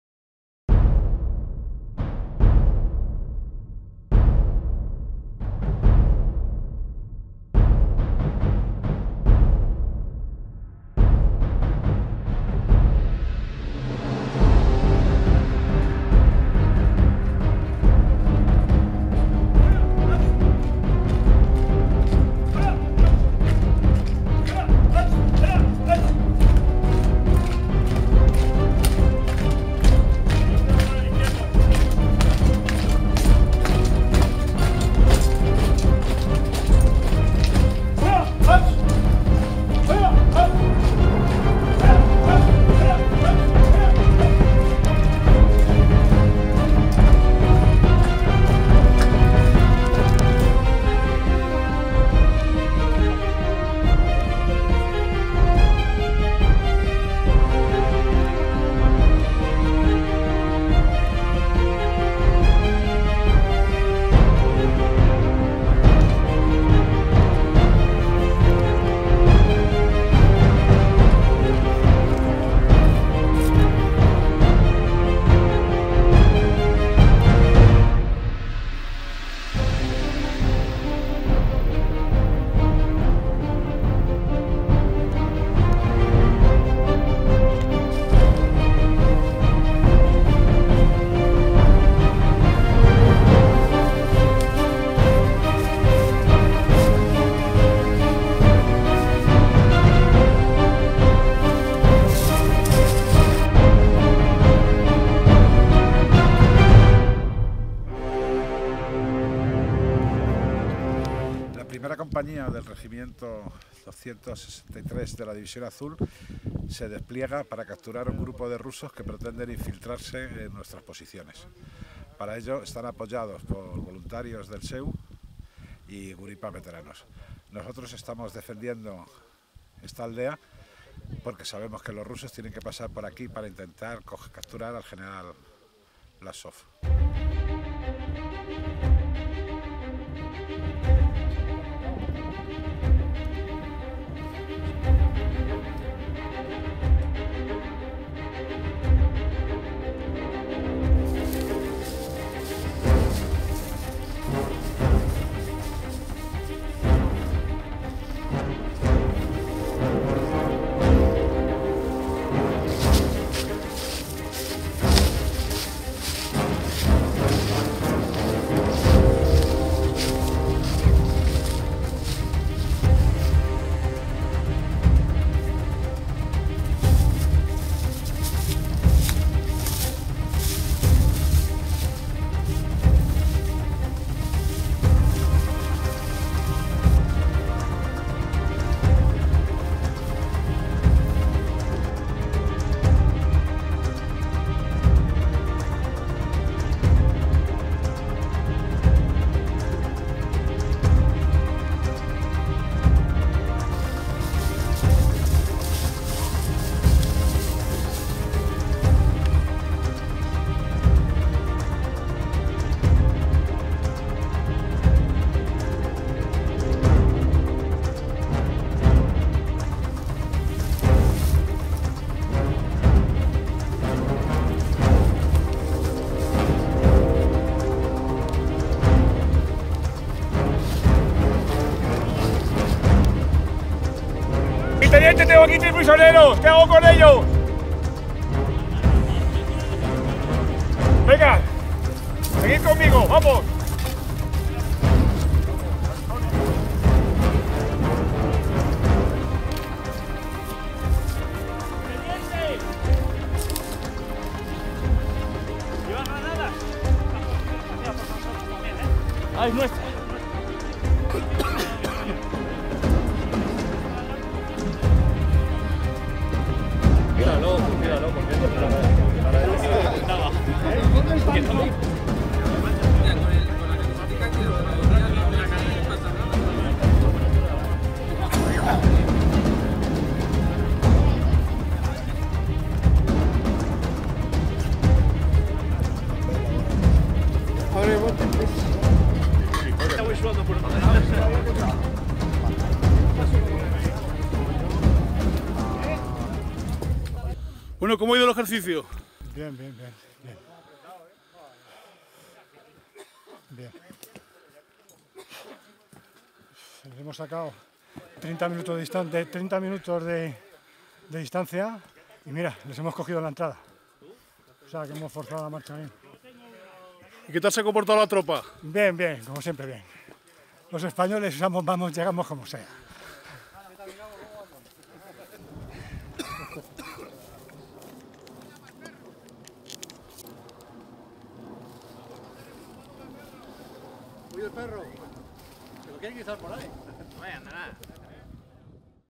Voljov 75: La captura del general Vlassov
Ejercicio táctico realizado por la Asociación División Azul de Reconstrucción Histórica Agrupación Capitán Urbano en Muriel (Guadalajara) donde se recreó la captura del general soviético Andrei Vlassov en la bolsa del Voljov en 1942.